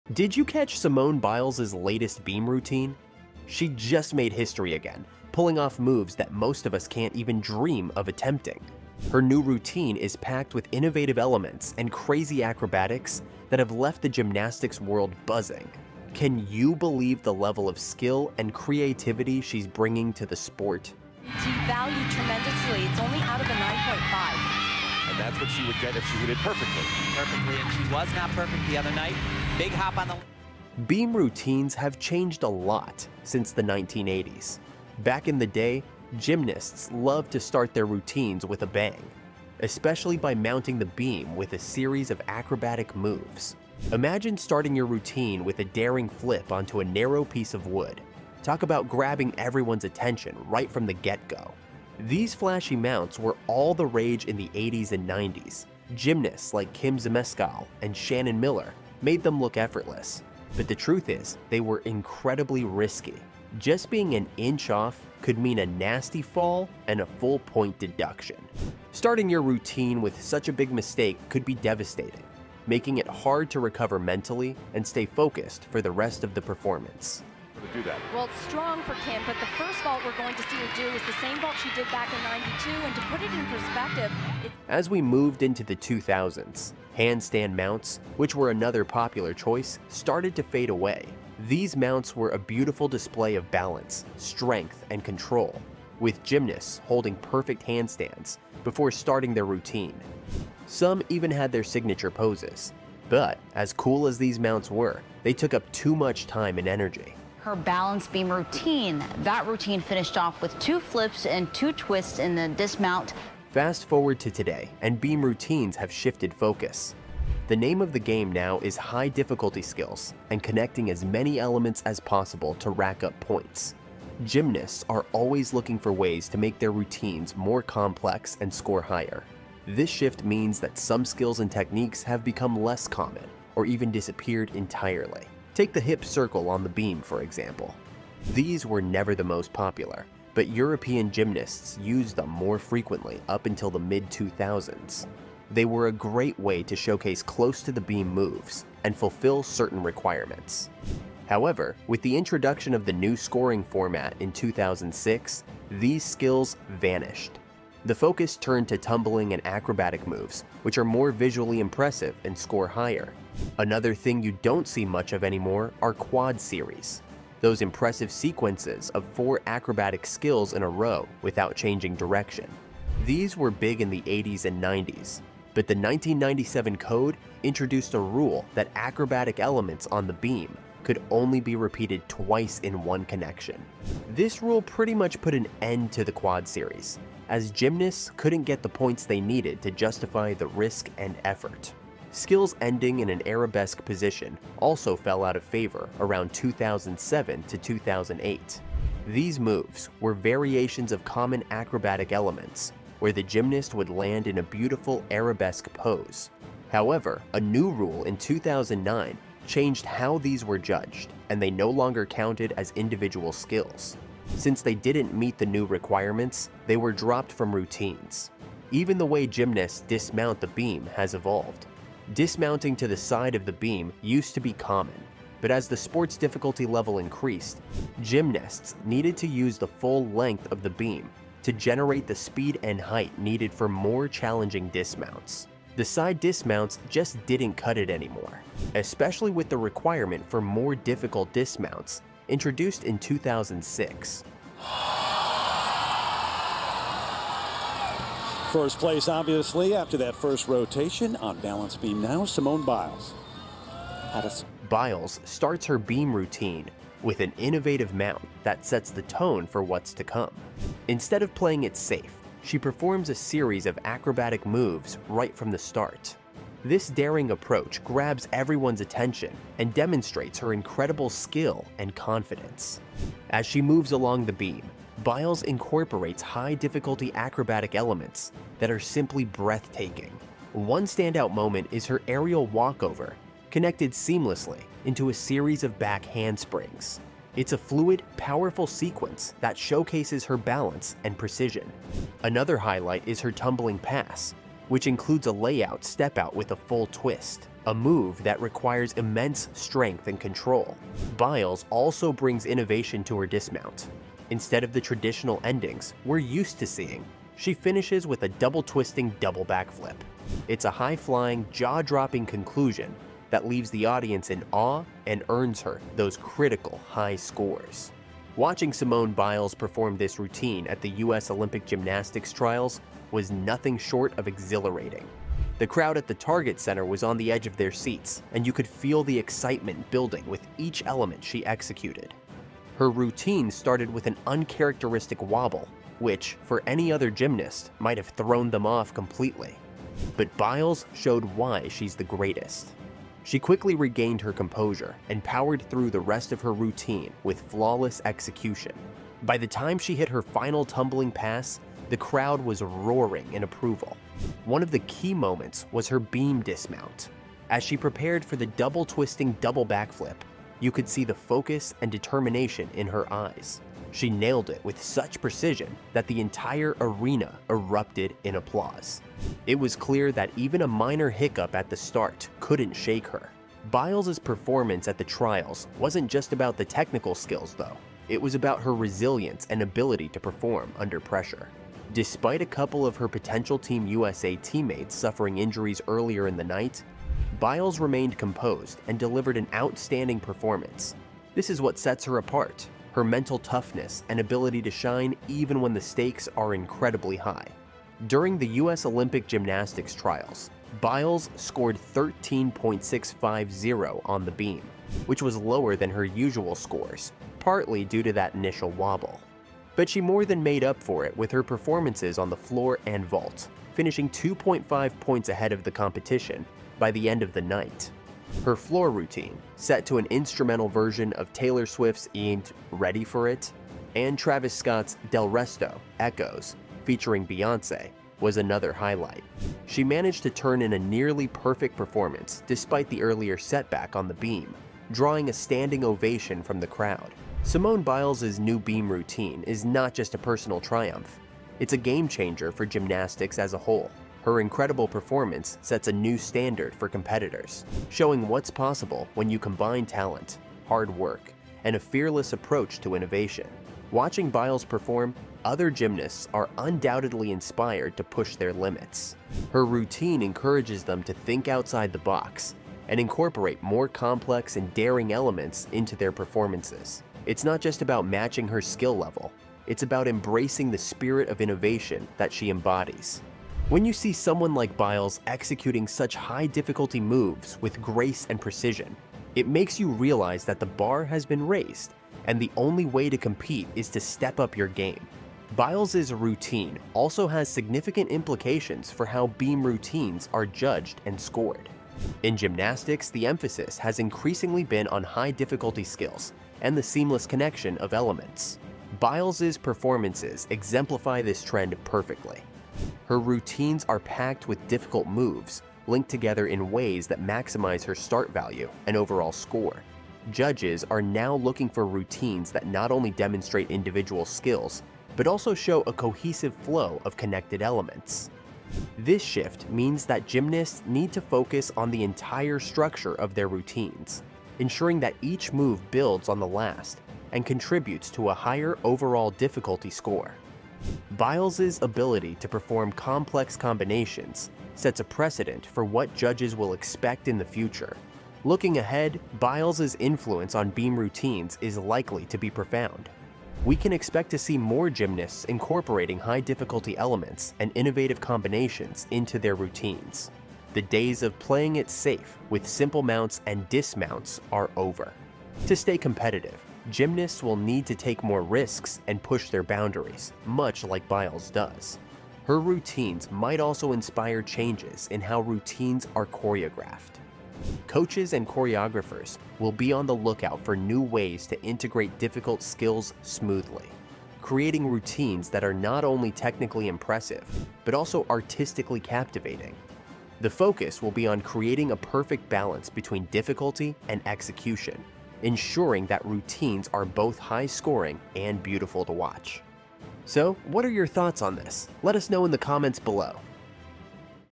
Simone Biles JUST MADE HISTORY With This NEW BEAM ROUTINE
Did you catch Simone Biles' latest beam routine? She just made history again, pulling off moves that most of us can't even dream of attempting. Her new routine is packed with innovative elements and crazy acrobatics that have left the gymnastics world buzzing. Can you believe the level of skill and creativity she's bringing to the sport?